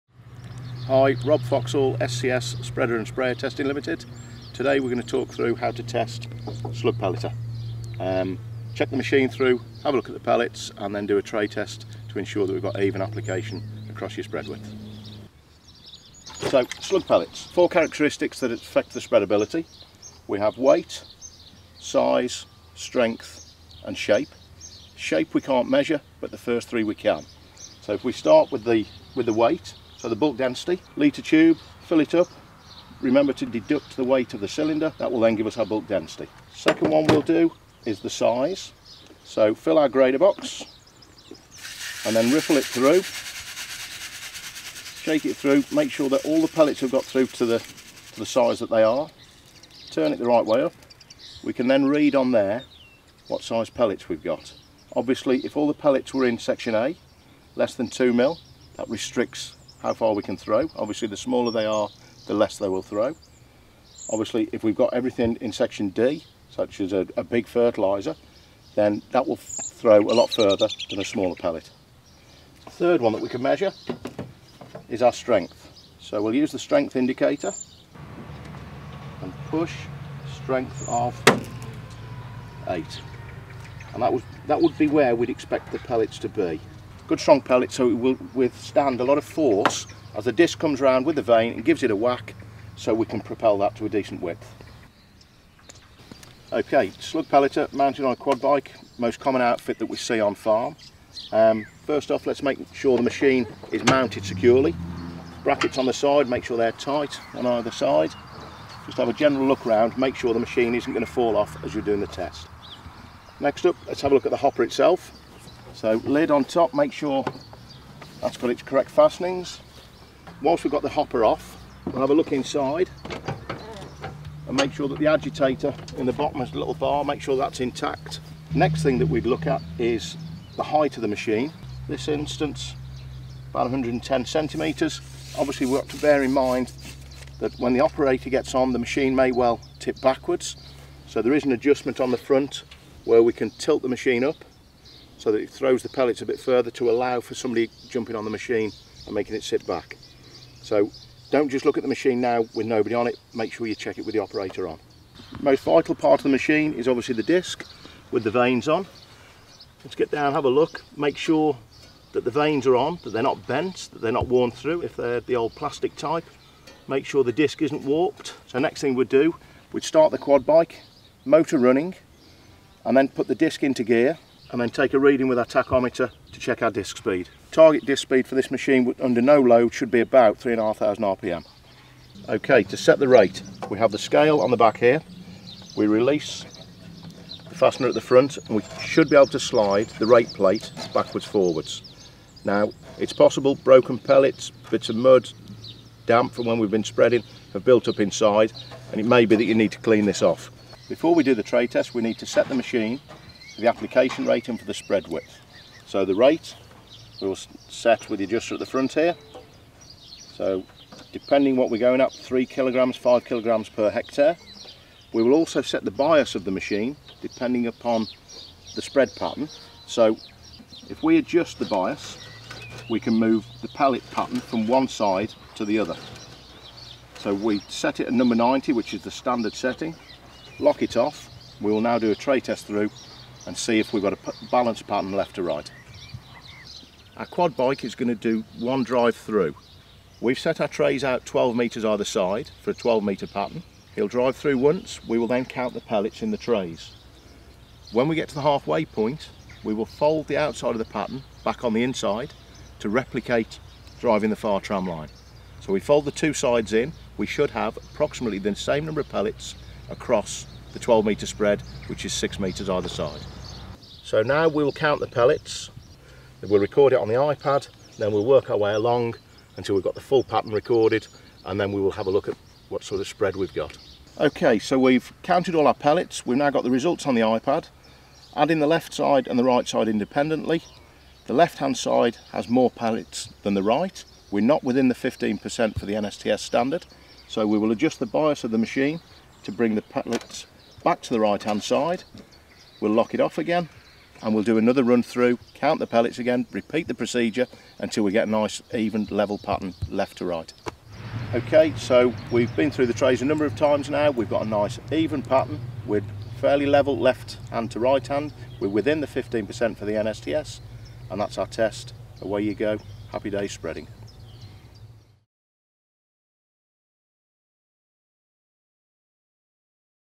How to calibrate a slug pellet spreader
Rob Foxall of SCS Spreader and Sprayer Ltd demonstrates how to test and calibrate a slug pellet spreader.